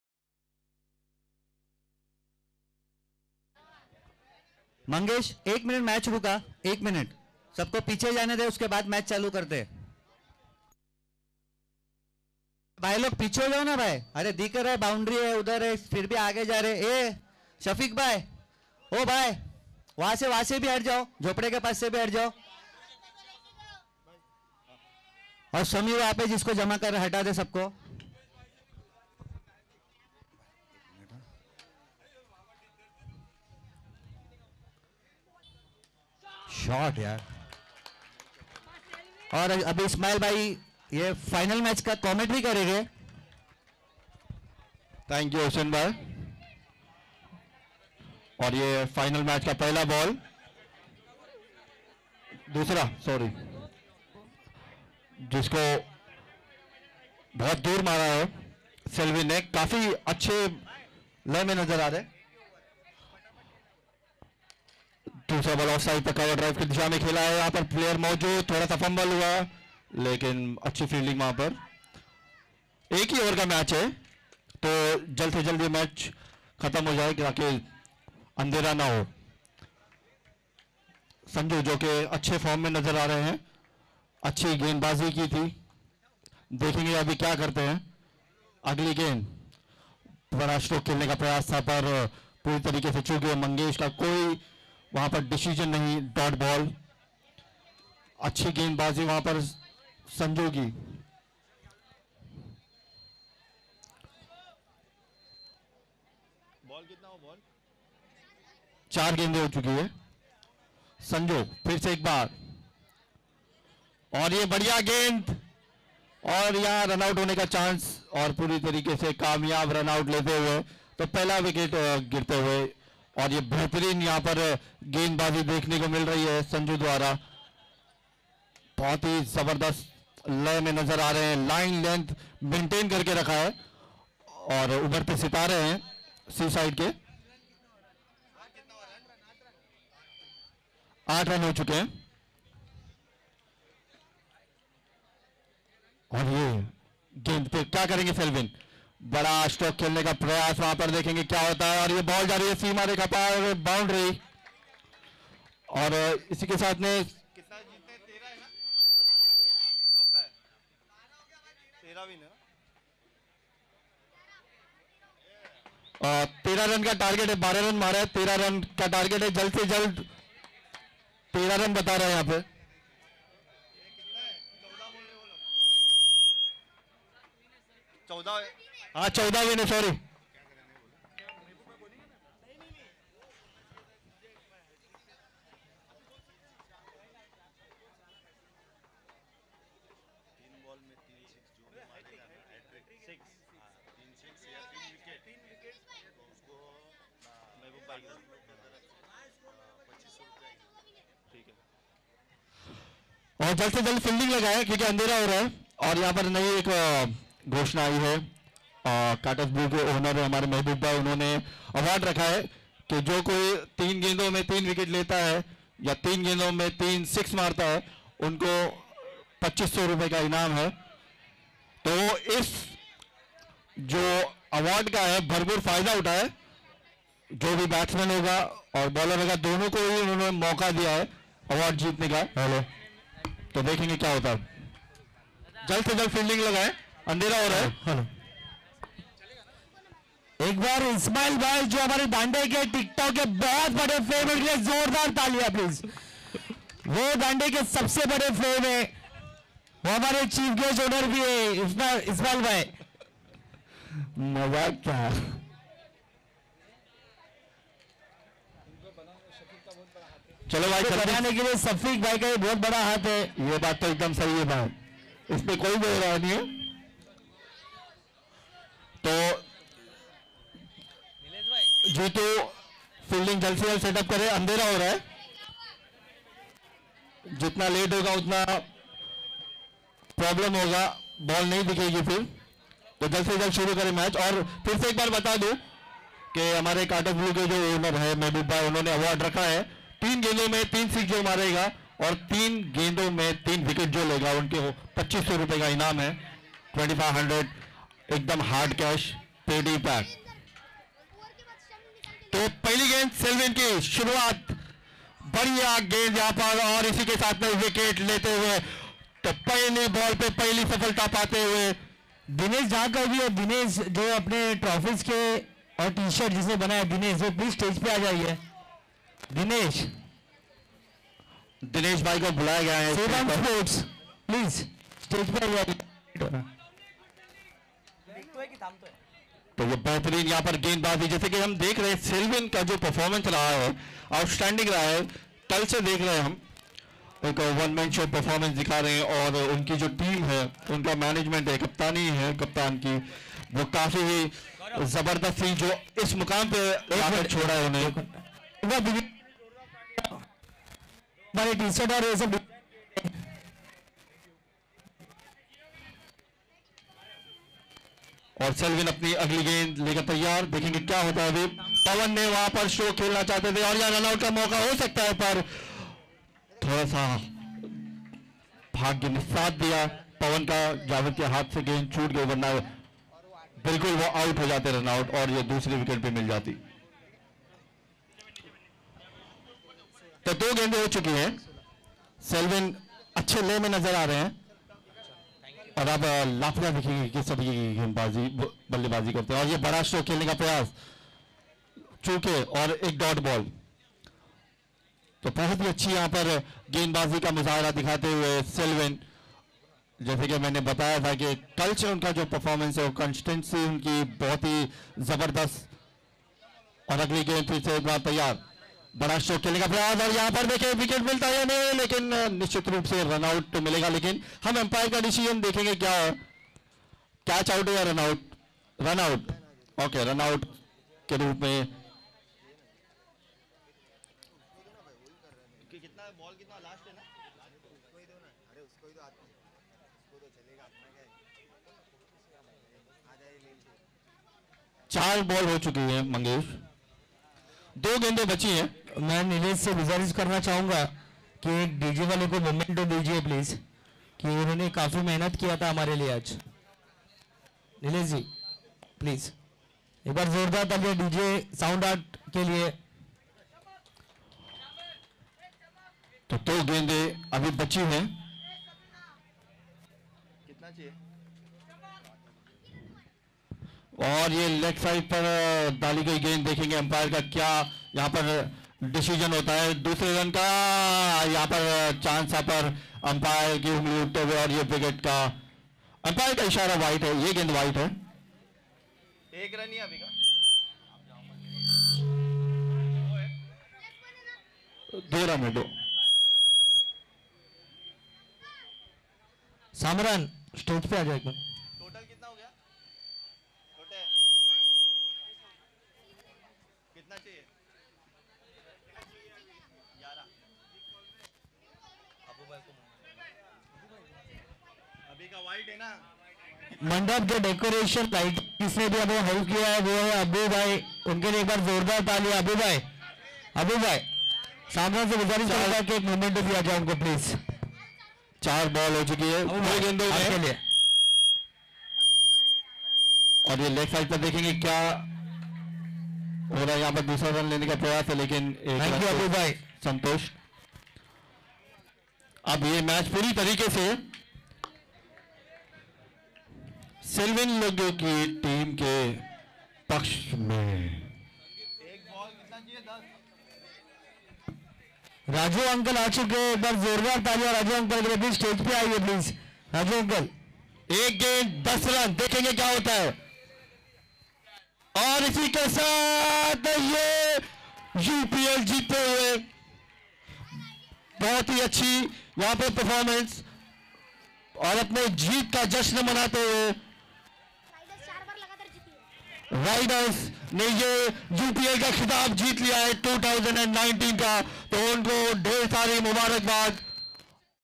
UPL 2019 | RIDERS vs WARRIORS | FINALS (Unity Premier League)
Watch the amazing action of RIDERS against the incredible WARRIORS in their Finale Match of the Unity Premier League 2019